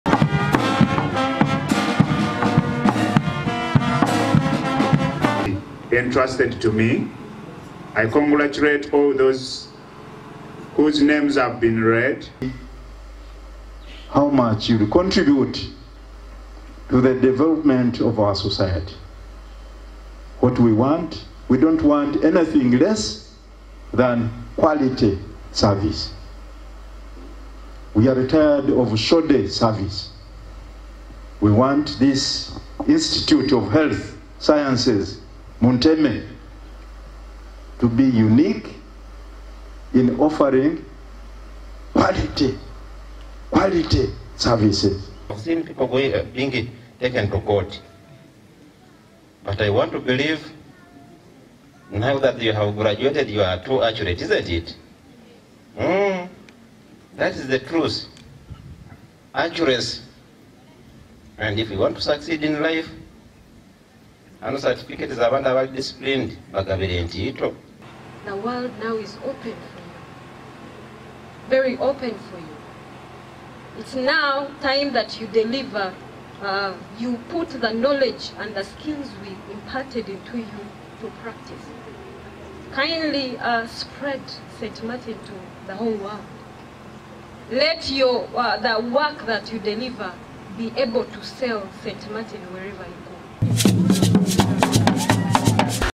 Bishop Kirabo tips lab technicians on ethics
The Bishop of Hoima Vincent Kirabo Amooti has cautioned laboratory technicians against giving inaccurate test results. Bishop Kirabo has encouraged laboratory technicians to always be mindful of giving accurate results during the course of their work. The Bishop made this call as presided over Mass to celebrate the first graduation of 613 graduands at St. Martin Institute of Health Sciences in Munteme, Kikuube District.